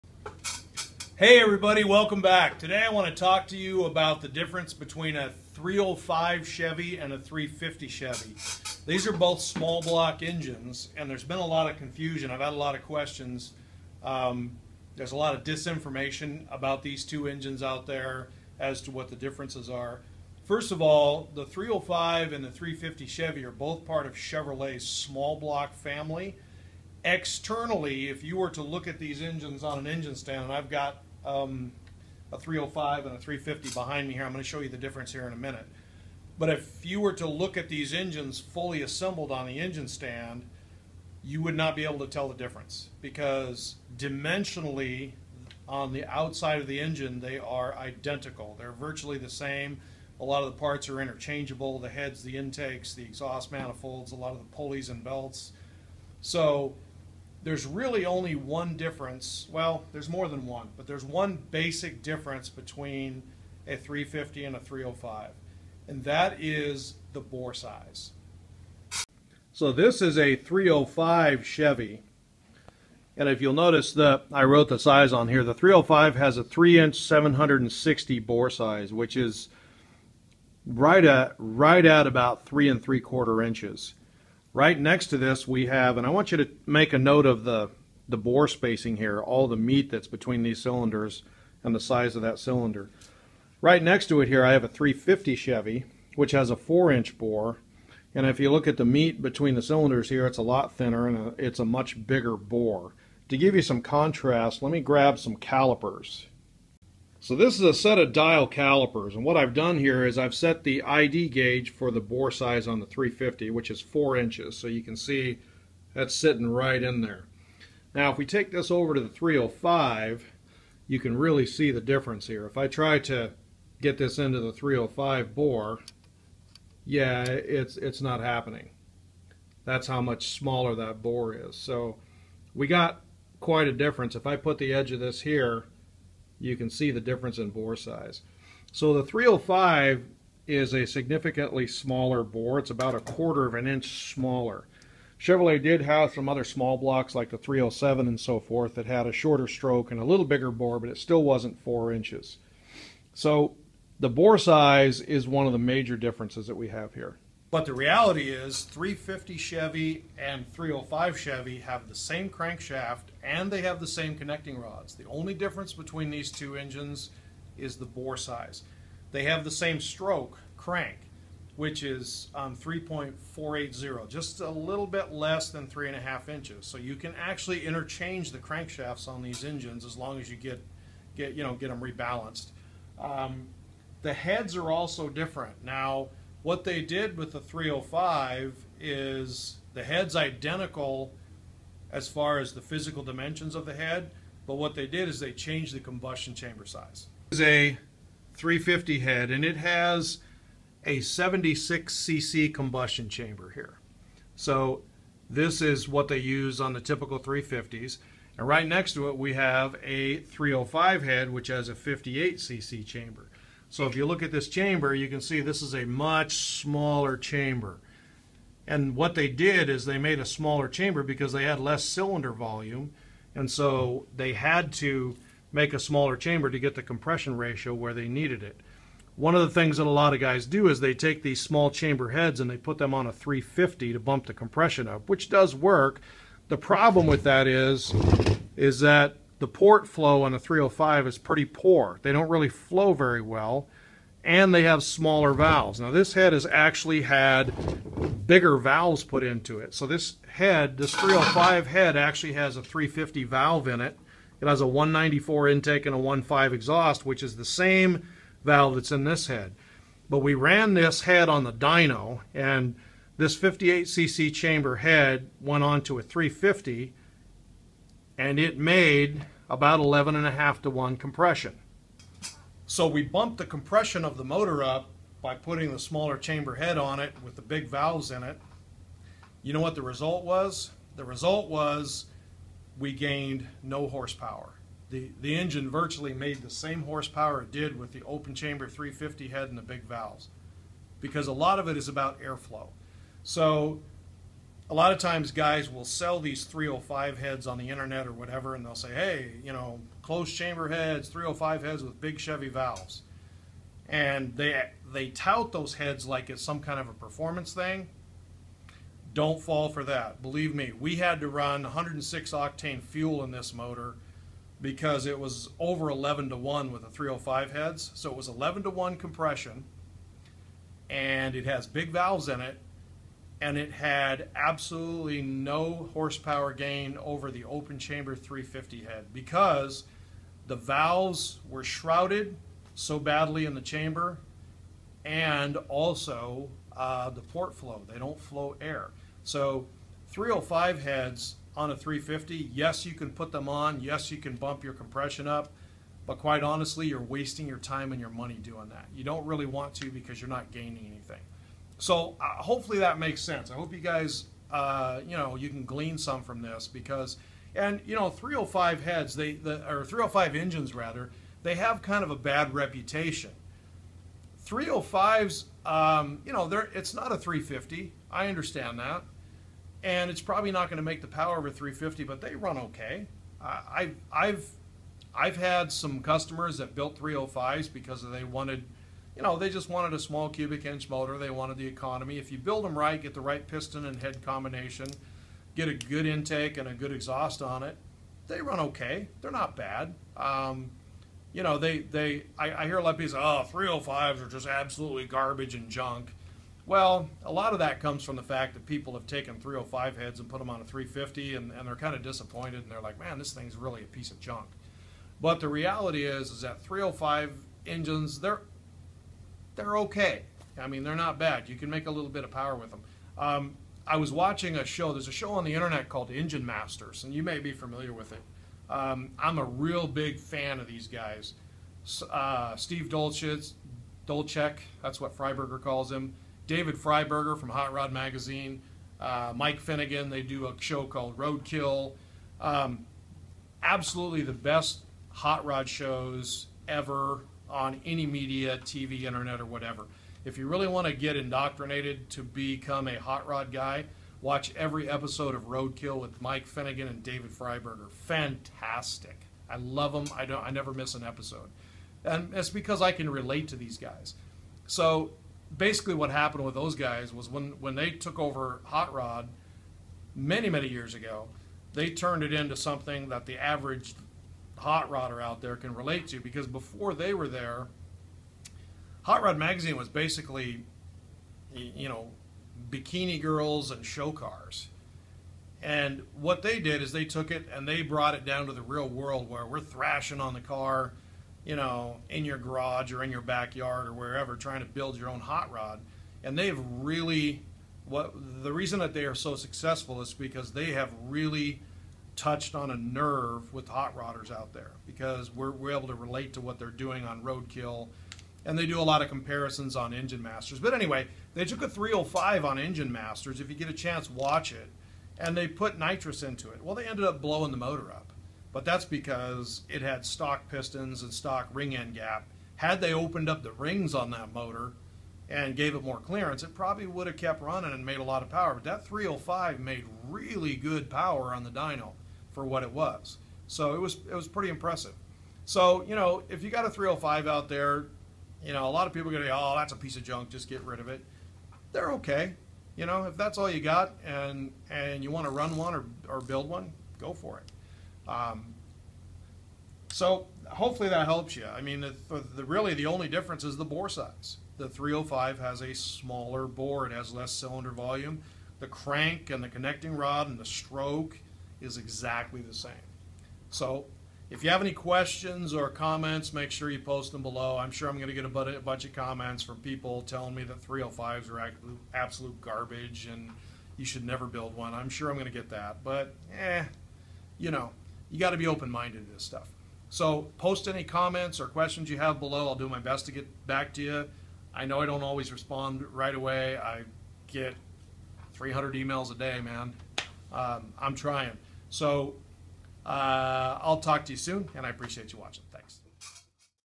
350 and 305 Chevy what's The Difference
What the the difference between a 305 and a 350 Chevy engines